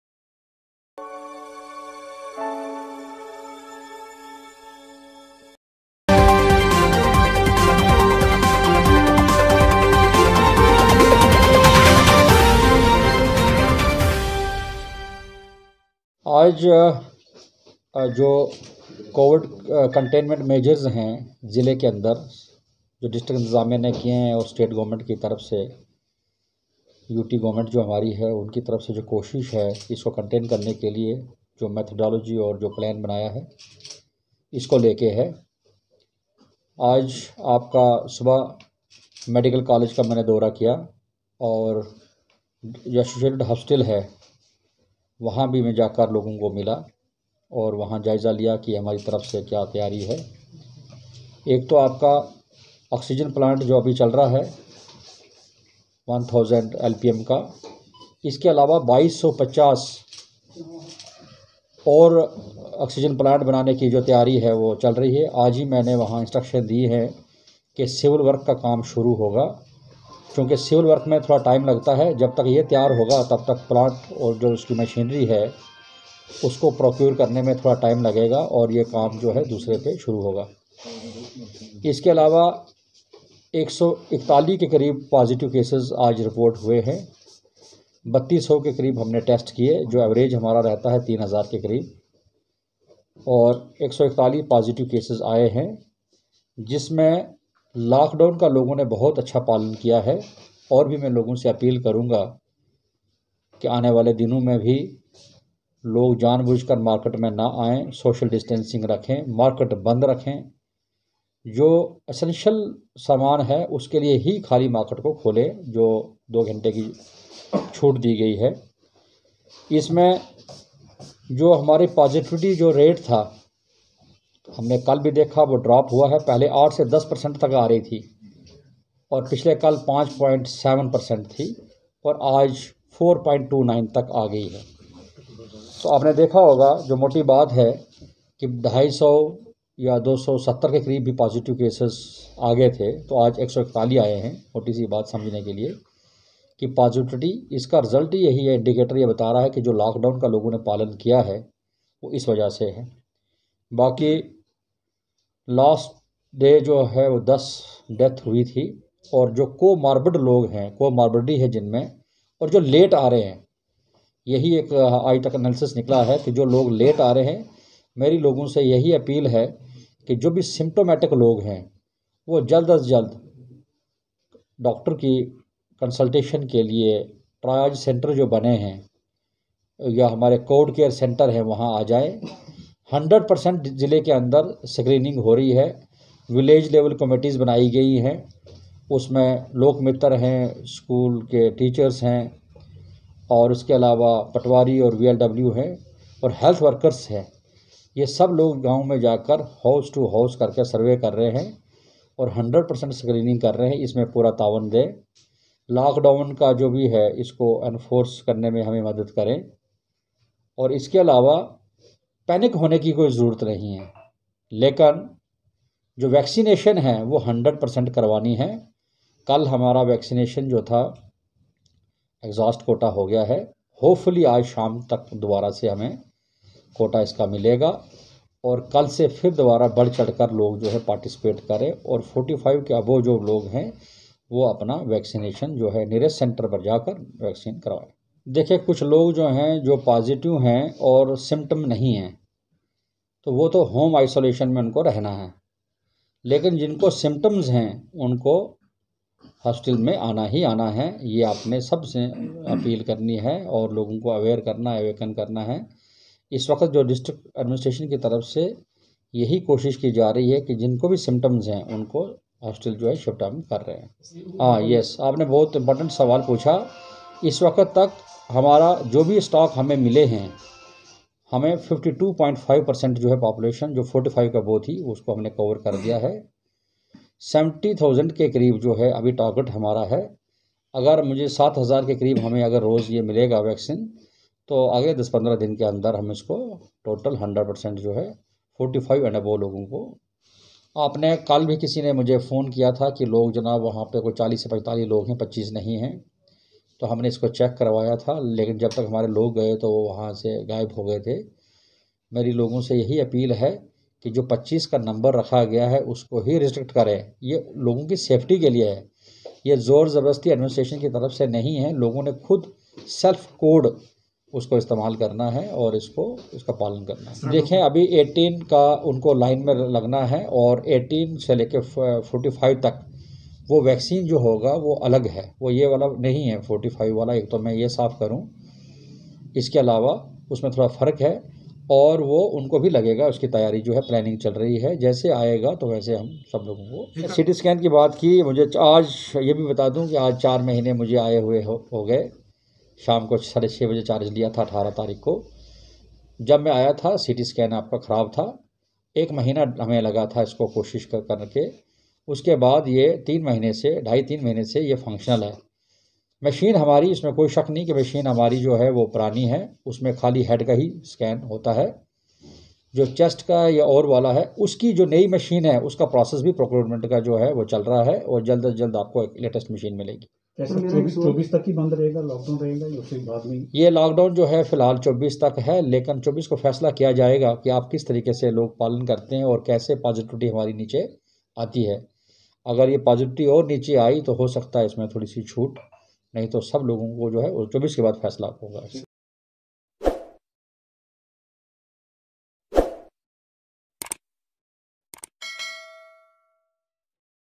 आज जिला कमिश्नर राजौरी राजेश कुमार शवन ने एक प्रेस वार्ता की उन्होंने कहा कि आज हम ने 3200 के करीब
आज जिला कमिश्नर राजौरी राजेश कुमार शवन ने एक प्रेस वार्ता की उन्होंने कहा कि आज हम ने 3200 के करीब सैंपलिंग की है जिस में 141केस पॉज़िटिव आए है, उन्होने कहा कि आज मैने जीएमसी राजौरी का बी दौरा किया और हमारा ऑक्सीजन प्लांट की तैयारी बड़ी जोरू से चल रही है,और हमारे कर्मचारी गर गर जा कर सैक्रिनिंग सर्वे कर रहे है, उन्होने जनता से अपील की कि जिस प्रकार पीछे आप ने हमारा साथ दिया आगे बी उसी प्रकार जरूरत है